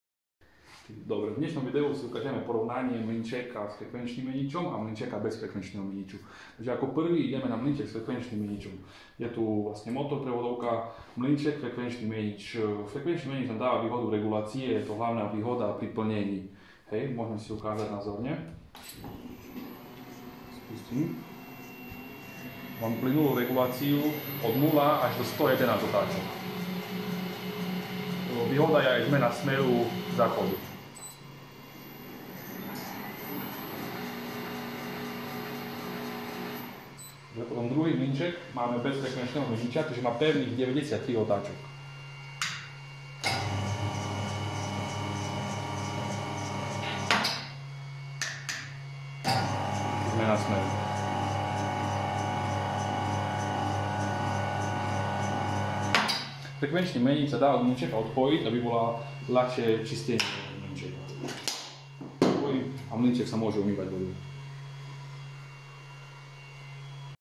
Elektrický mlynček na mäso PORKERT 32 a frekvenčný menič INVT GD20
KOMPONENTY NA HORE UVEDENOM MODROM LINKU

 Elektrický mlynček  na mäso PORKERT 32 s frekvenčným meničom INVT od firmy MTM Electric Motors s.r.o. pre plynulú reguláciu otáčok